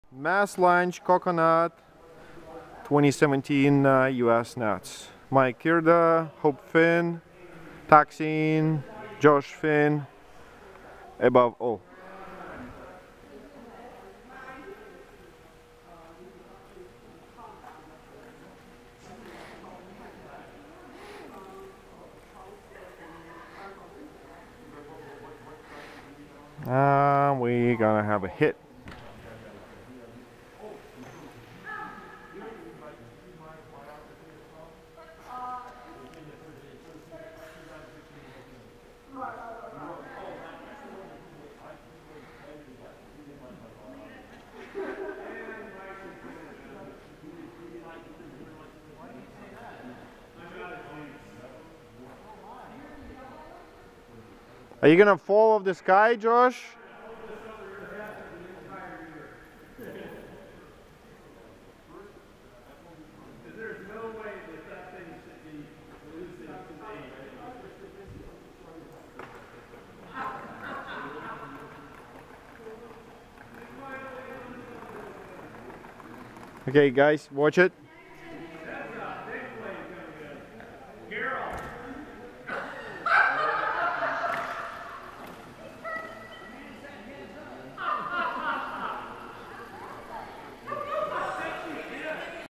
Coconut mass launch USIC 2017
The 2017 US NATS: Coconut Mass Launch.